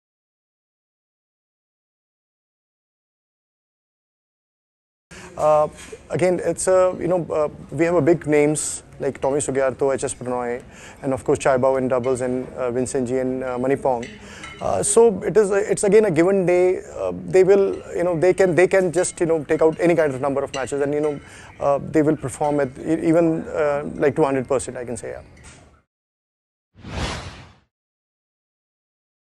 Ajay Kanwar, Head Coach, Delhi Dashers - Video
Ajay Kanwar, Head Coach, Delhi Dashers, hints at the element of surprise in his star-studded team. Watch him speak as the PBL Season IV heads towards its final stage.